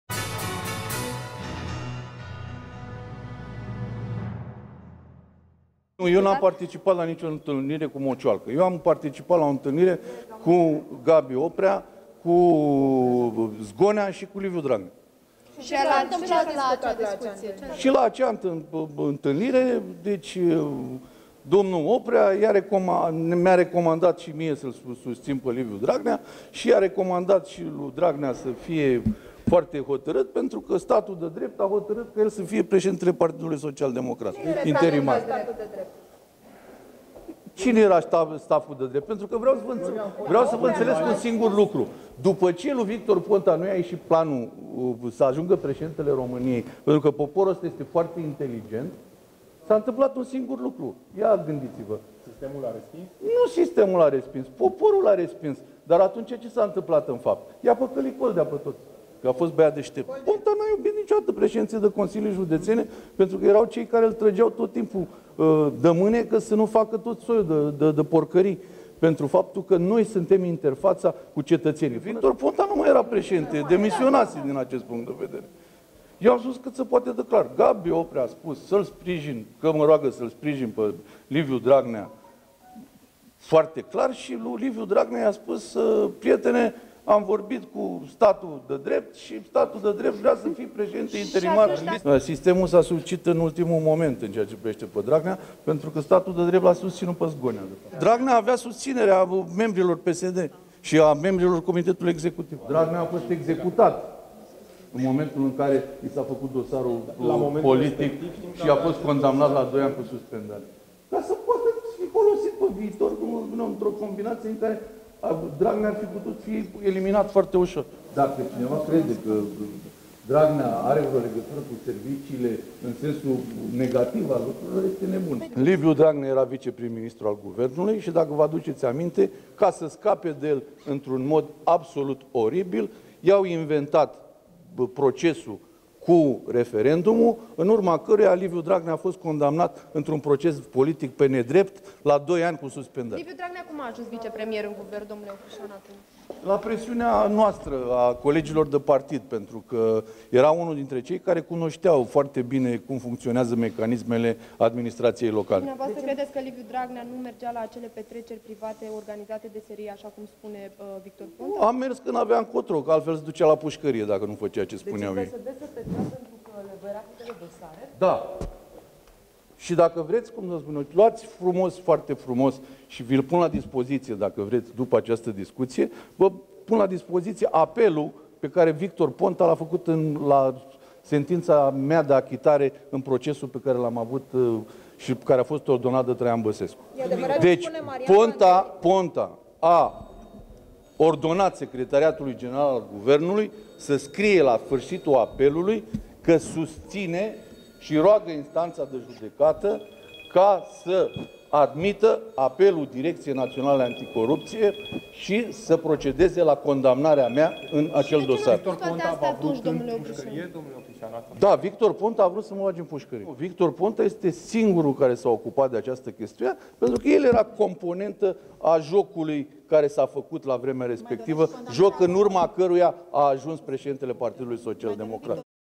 Oprișan: Oprea mi-a recomandat să-l susțin pe Dragnea ca președinte interimar al PSD
Liderul PSD Vrancea, Marian Oprişan, a povestit luni că a participat la o întâlnire cu fostul preşedinte al UNPR Gabriel Oprea, care i-a recomandat să îl susţină pe Liviu Dragnea pentru funcţia de preşedinte interimar al social-democraţilor, după ce Victor Ponta îşi dăduse demisia din această funcţie.Oprişan spune că "statul paralel" l-a susţinut mai întâi pe Valeriu Zgonea pentru această funcţie, după care "s-a sucit", pentru că Liviu Dragnea era sprijinit de marea masă a partidului, în special de liderii de filiale teritoriale.